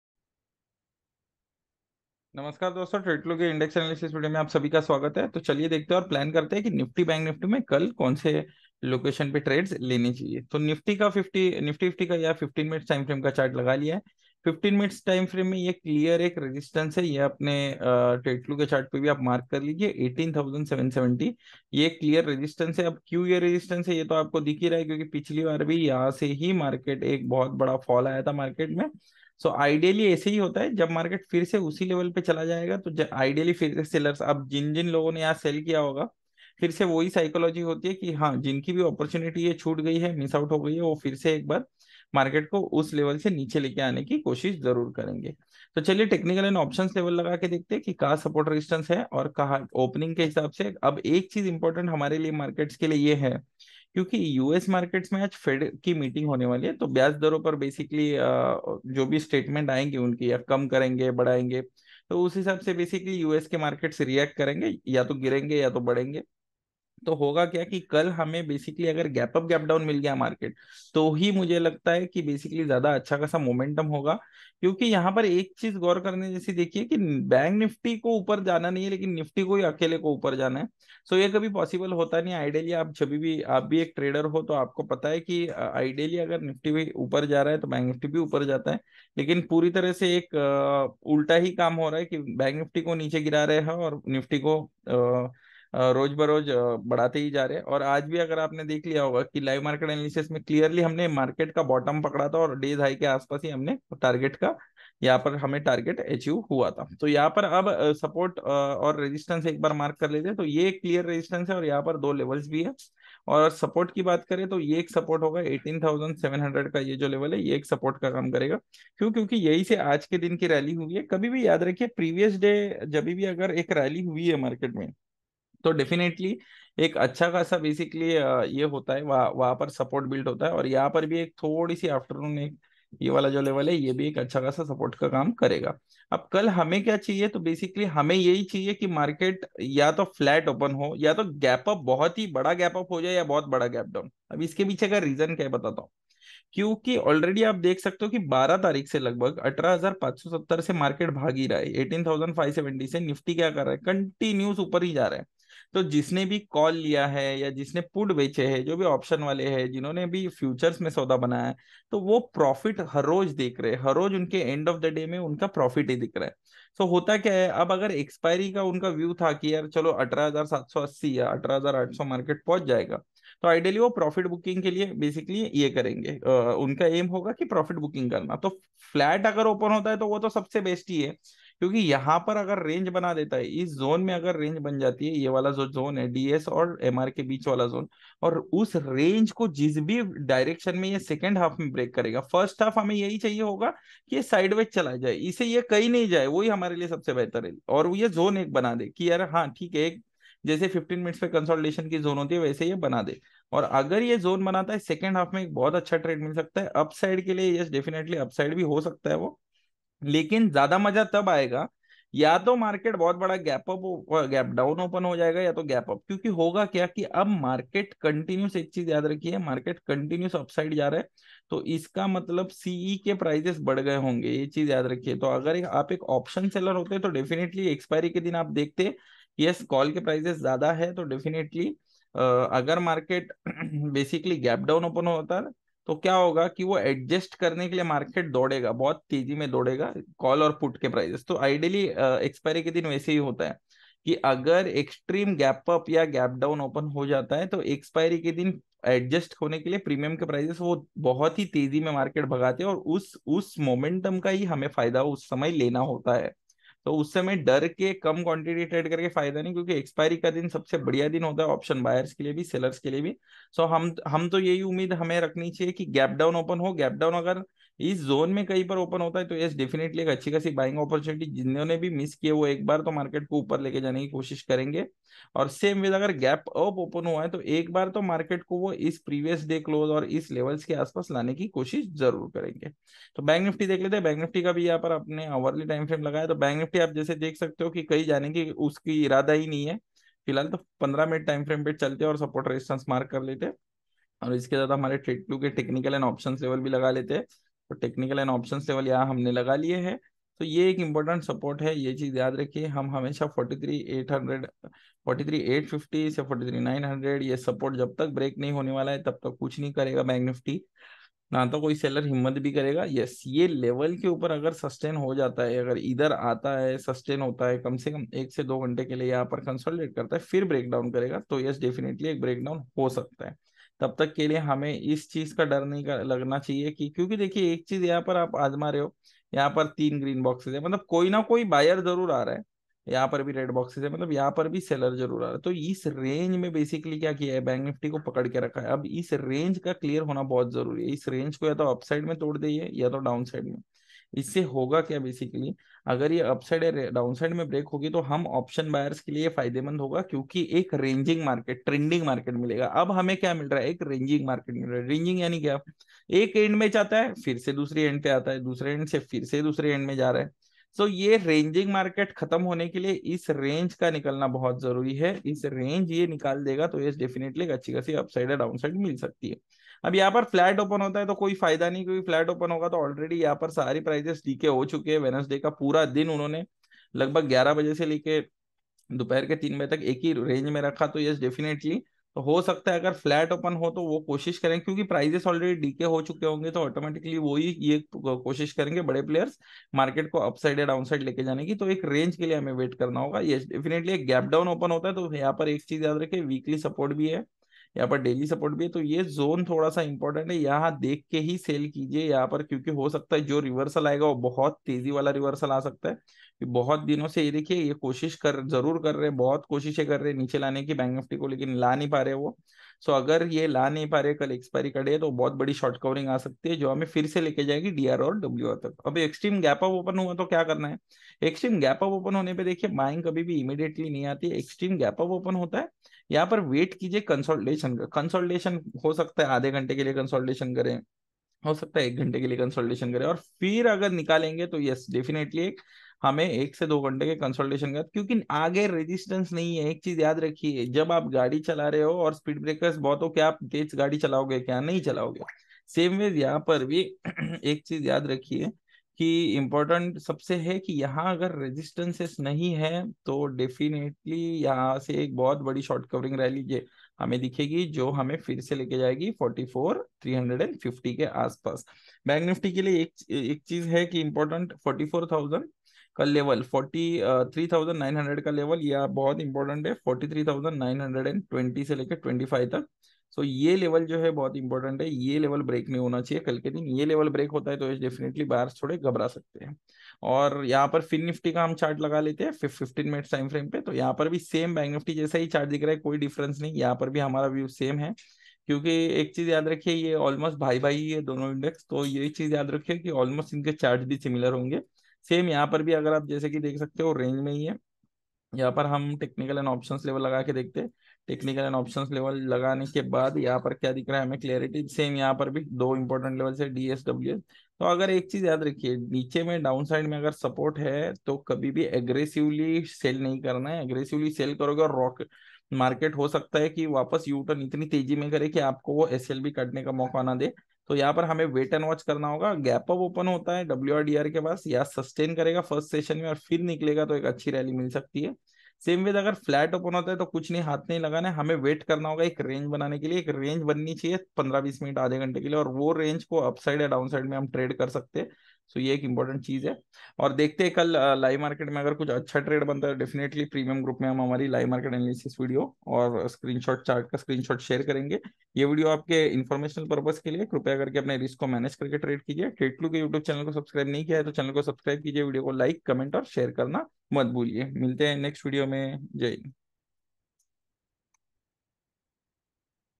Market Commentary 15Jun2023 🔔📉📈🔔
In this video we are discussing important levels for NIFTY-BANKNIFTY on TradeClue. We also analyze what our trading plan should be for today.

We focus on how TradeClue can be used to improvise our trading by understanding basic price action concepts..

For any assistance reach out to our support team.

SEBI REGISTERED ANALYST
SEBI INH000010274